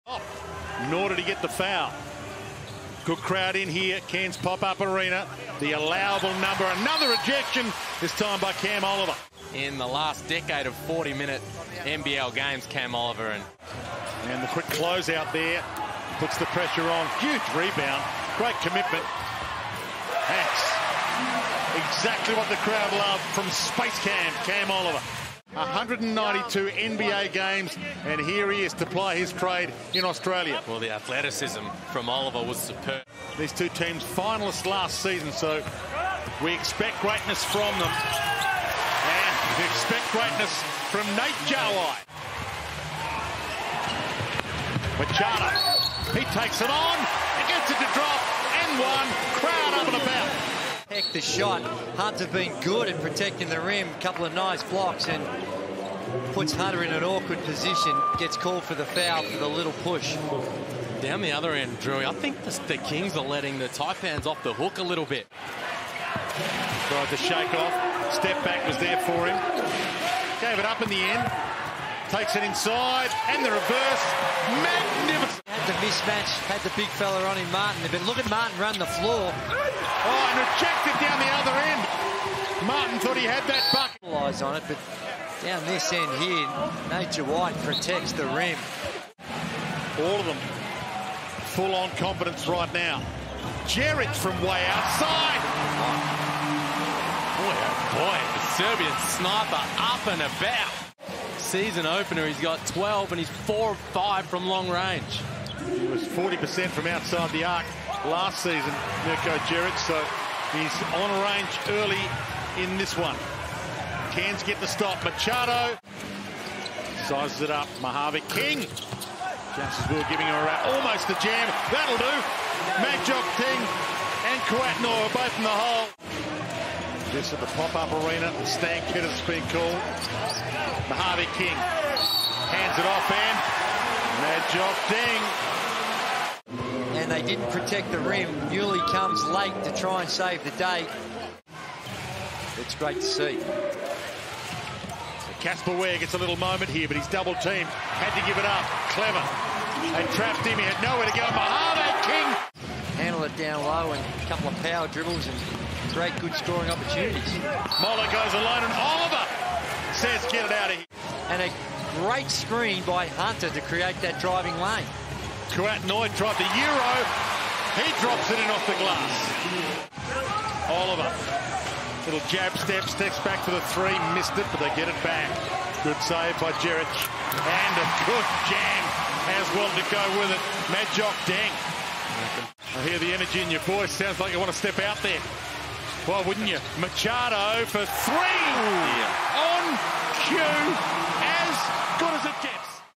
Cairns Taipans v Sydney Kings NBL highlights (Round 1, 2020/2021)
All the highlights from the win over the Sydney Kings.